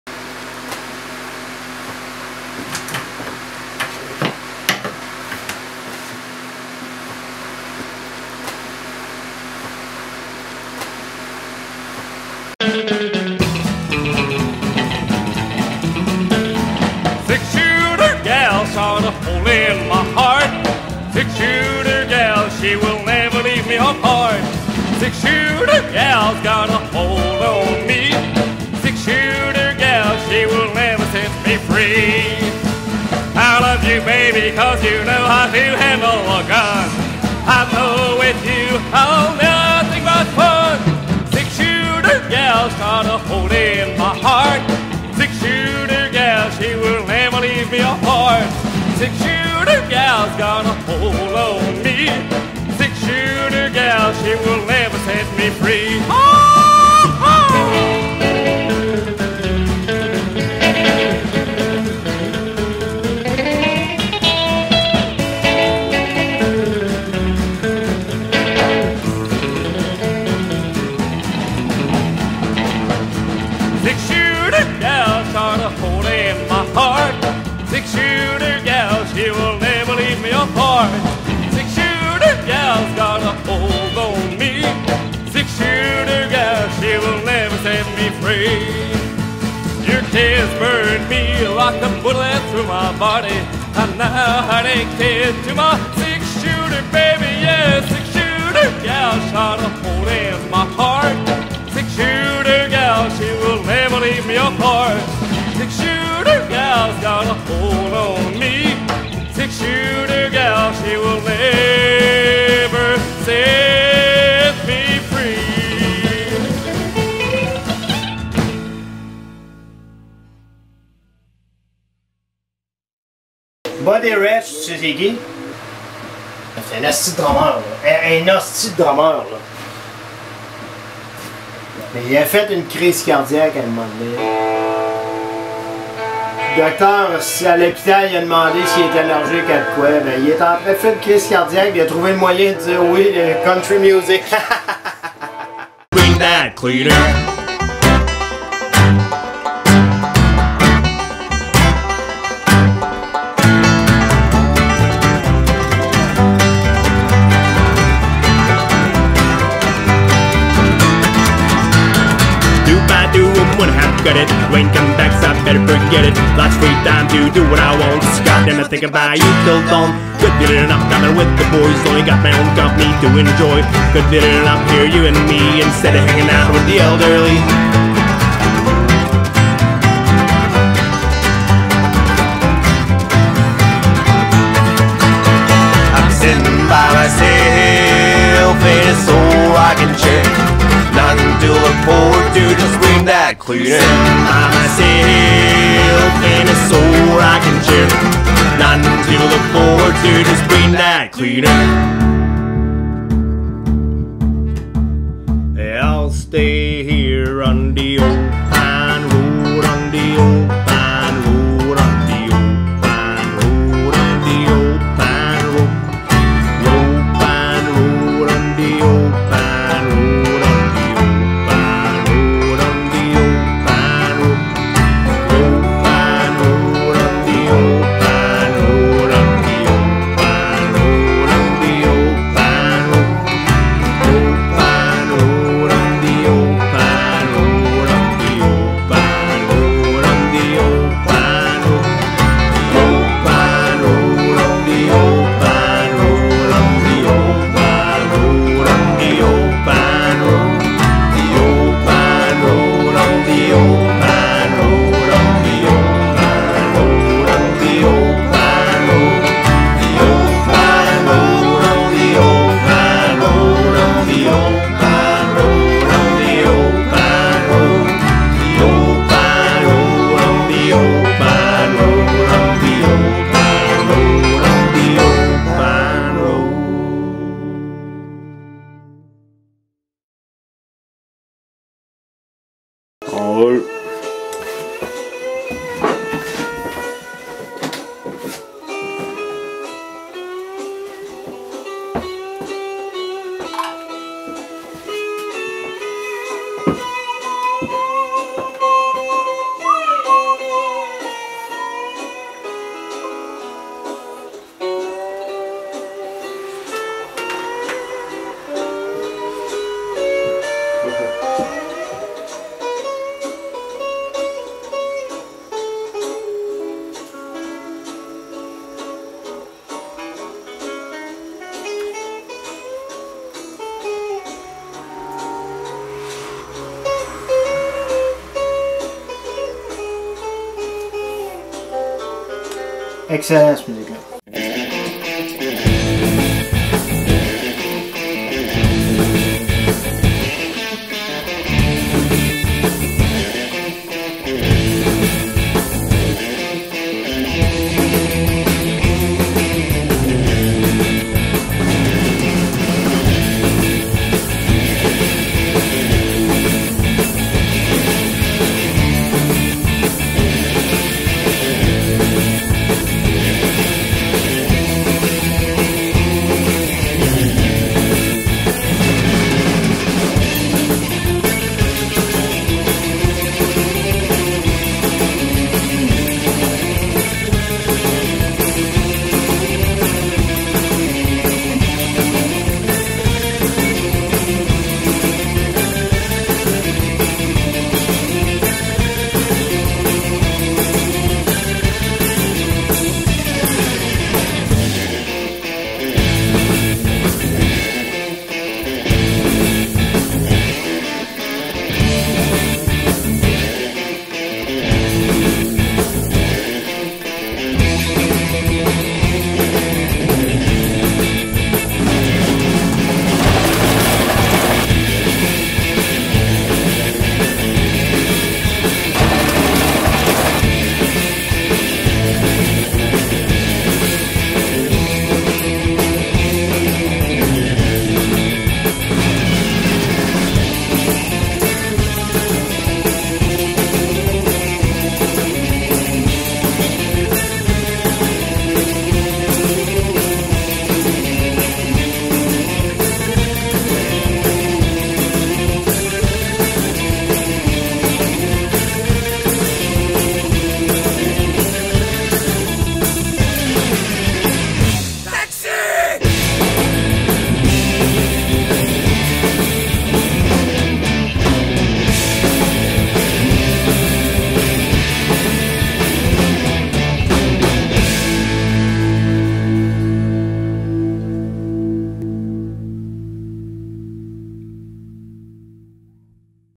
"Soiree Western"
A short film featuring the workmanship of sign painter Pierre Tardif to the music of the bands he crafts a fantastic showcard for.